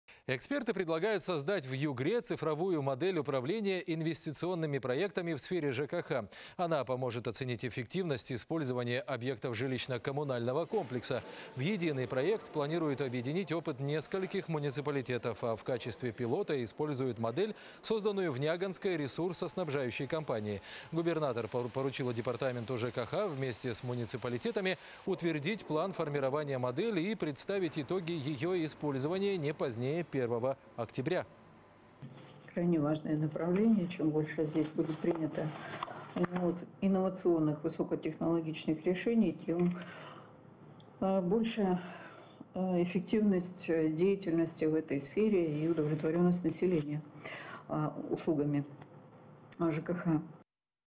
Цифровую модель управления инвестиционными проектами в сфере ЖКХ внедрят в Югре
Эксперты предлагают создать в Югре цифровую модель управления инвестиционными проектами в сфере ЖКХ. Она поможет оценить эффективность использования объектов жилищно-коммунального комплекса.